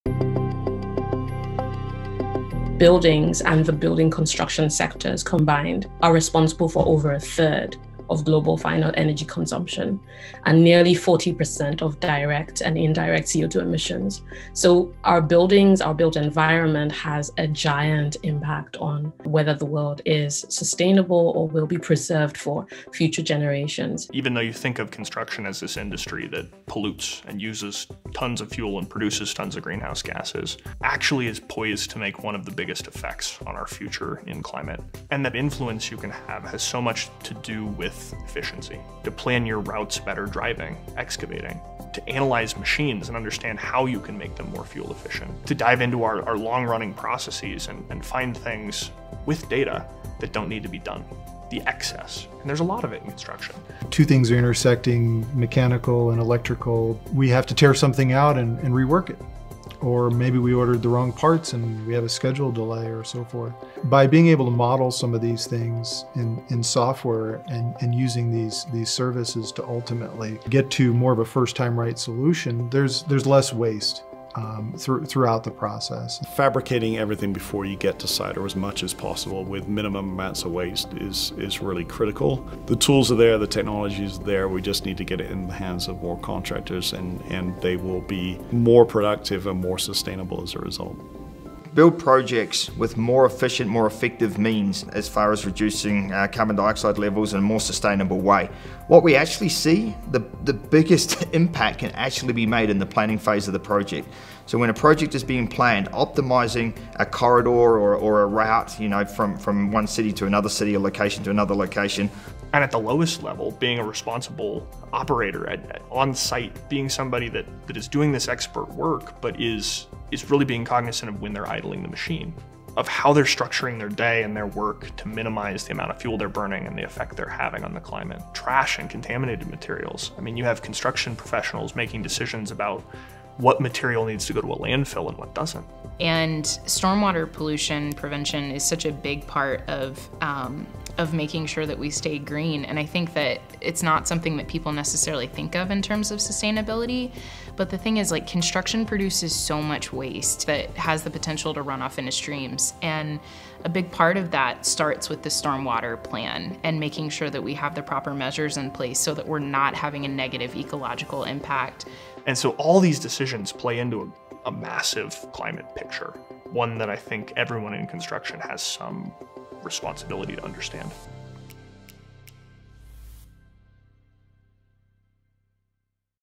Sustainability in Construction: During the Construction Phase
We sat down with tech & construction leaders to get their thoughts on sustainability in construction, now and in the future. In this video, we focus on the efforts that can be done during the construction phase to 'build green'.